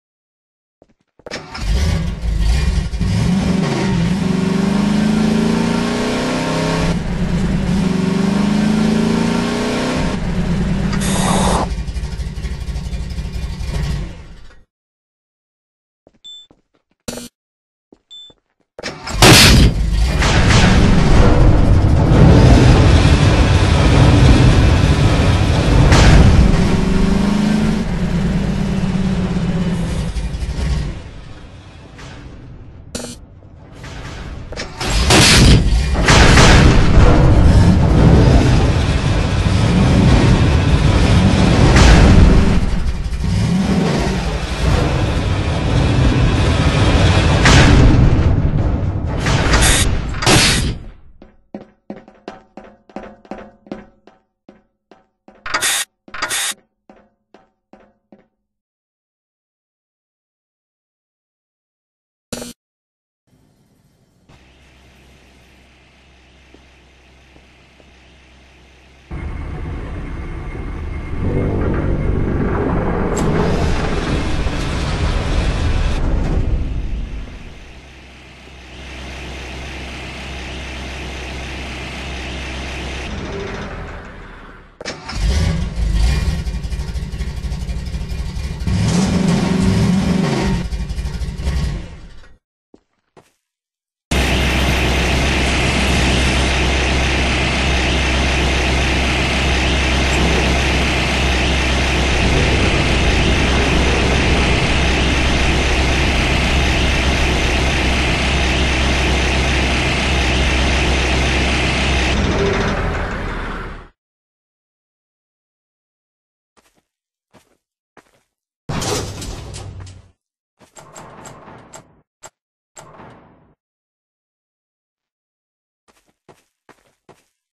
Spuzzbunk4
Jazzed up the elevator and added a new complex door at the bottom